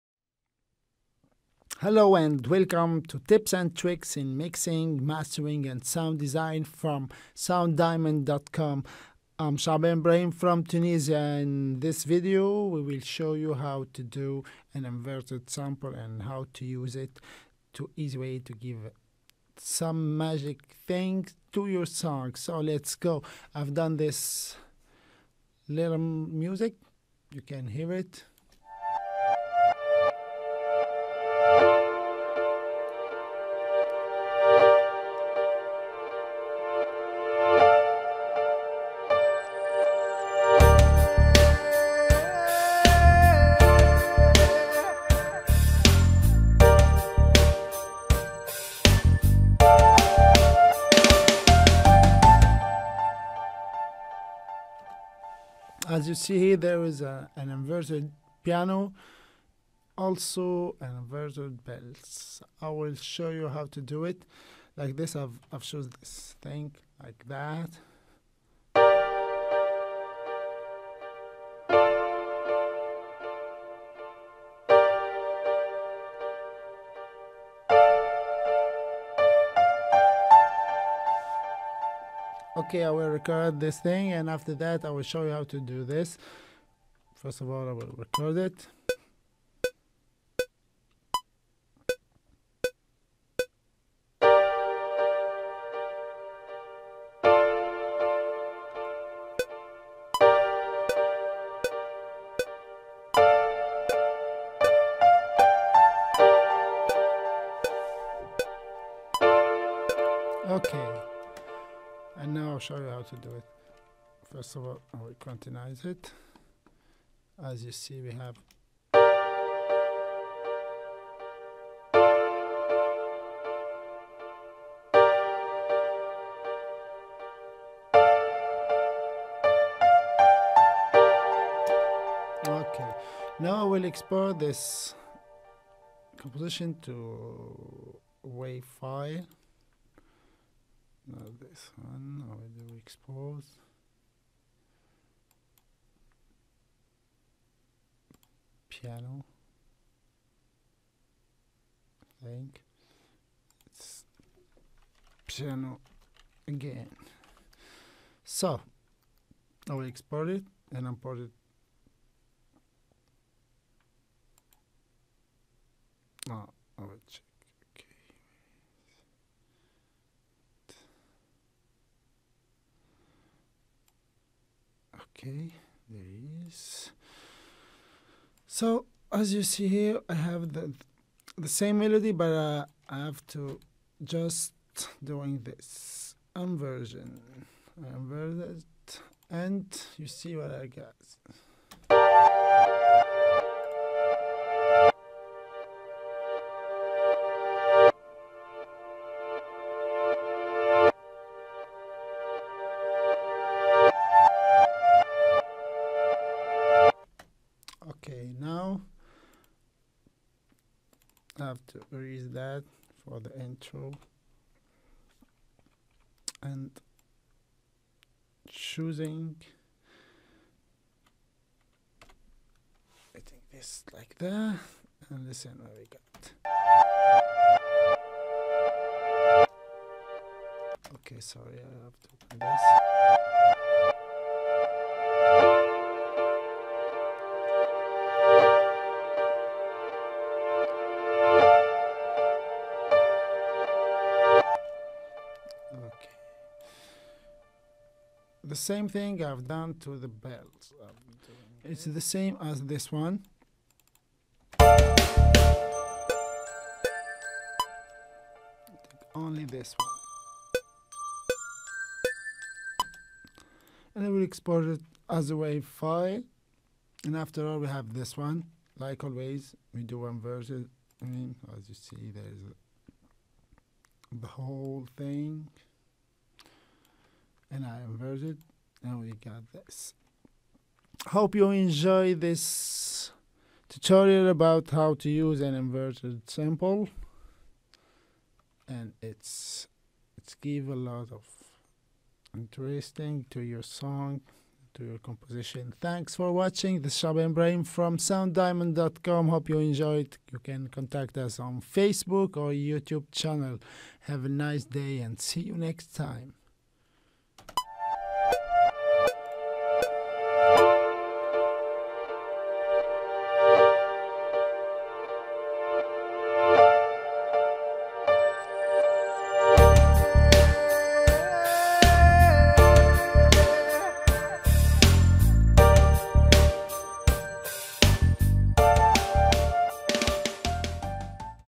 Creating inverted samples
in this video we will show you how to inverted samples or melodies
it is easy and useful for many persons that can figure out how to do it
by chaabane ibrahim
in tips and tricks mixing mastering ans sound design